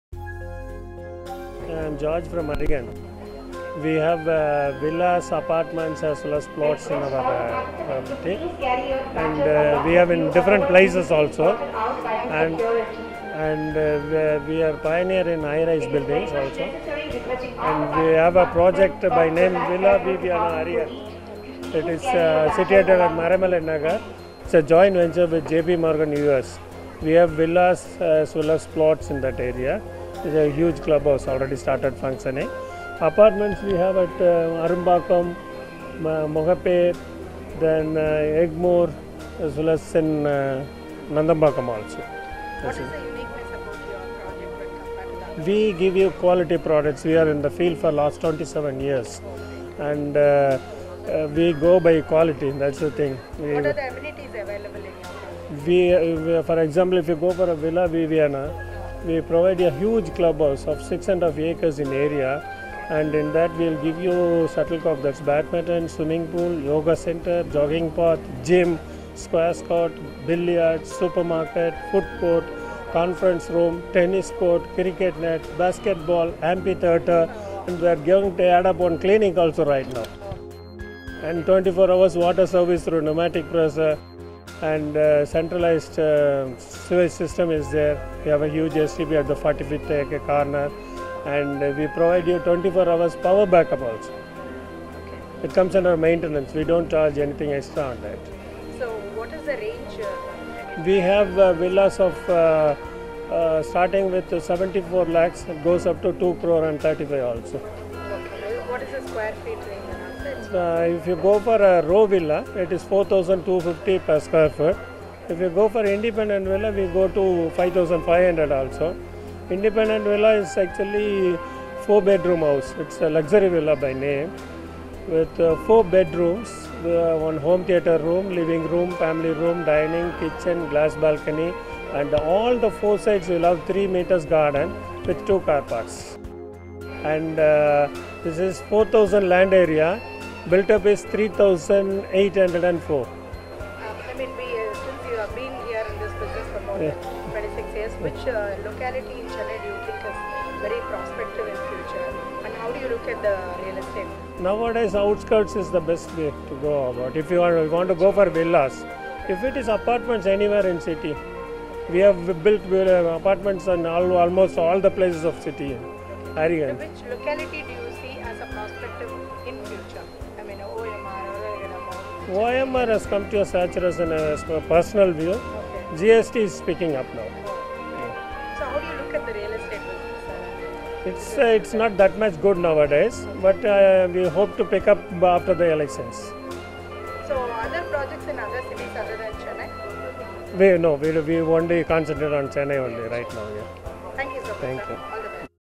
Sulekha interviews Arihant @ FairPro 2014
Mr. George from Arihant Housing Ltd speaks to Sulekha at the FairPro 2014 exhibition held at Chennai Trade Centre from 21 Feb - 23 Feb.